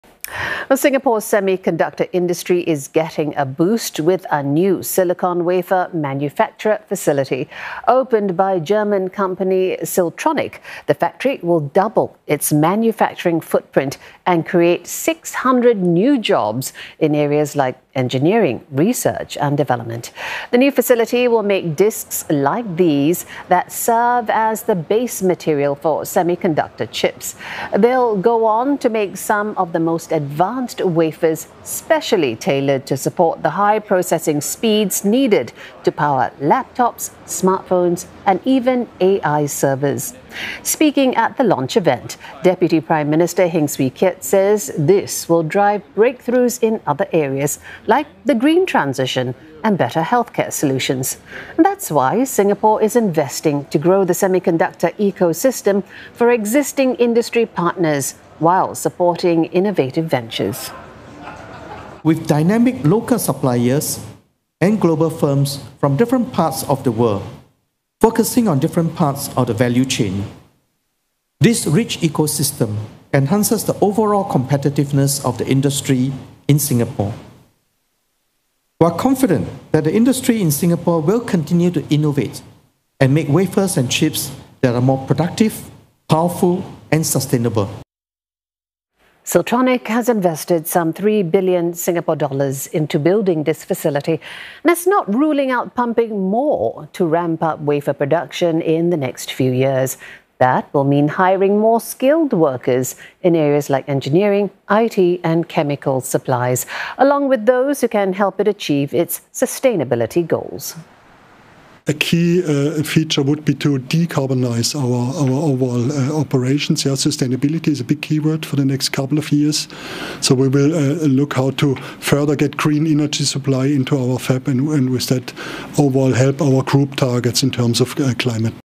Siltronic's new $3b wafer manufacturing facility in Singapore to create 600 skilled jobs
Singapore's semiconductor industry is getting a boost, with a new $3-billion silicon wafer manufacturing facility. Opened by Germany's Siltronic, the factory will double its manufacturing footprint and create 600 jobs in areas like engineering, research and development. At the launch event, Deputy Prime Minister Heng Swee Keat said this will drive breakthroughs in other areas like the green transition and better healthcare solutions. That is why Singapore is investing to grow the semiconductor ecosystem for existing industry partners, while supporting innovative ventures.